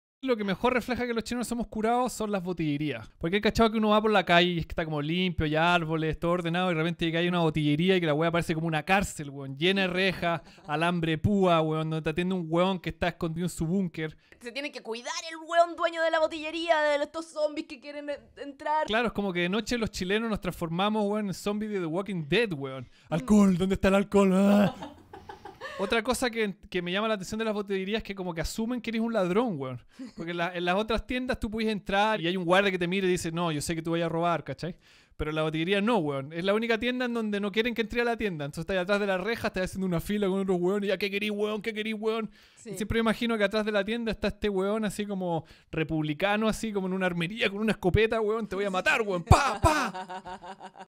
¿Somos curaditos los chilenos? 🤔🍷
🍾¿Crees que somos más borrachines que en otros países? 🍷